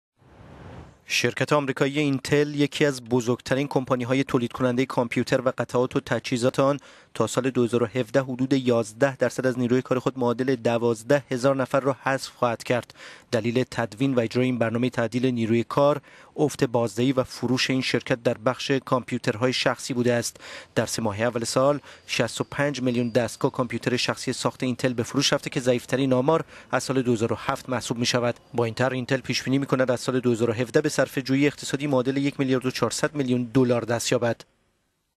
تعدیل نیروی کار وسیع در شرکت کامپیوتری «اینتل» - economy
شرکت آمریکایی اینتل، یکی از بزرگترین کمپانی های تولیدکننده کامپیوتر و قطعات و تجهیزات آن تا سال ۲۰۱۷ حدود ۱۱ درصد از نیروی کار خود معادل ۱۲ هزار نفر را حذف خواهد کرد.

دلیل تدوین و اجرای این برنامه تعدیل نیروی کار افت بازدهی و فروش این شرکت در بخش کامپیوترهای شخصی بوده است. در سه ماهه اول سال ۶۵ میلیون دستگاه کامپیوتر شخصی ساخت اینتل به فروش رفته که ضعیف ترین آمار از سال ۲۰۰۷ محسوب می شود.

با این طرح اینتل پیش بینی می کند از سال ۲۰۱۷ به صرفه جویی اقتصادی معادل ۱ میلیارد و ۴۰۰ میلیون دلار در سال…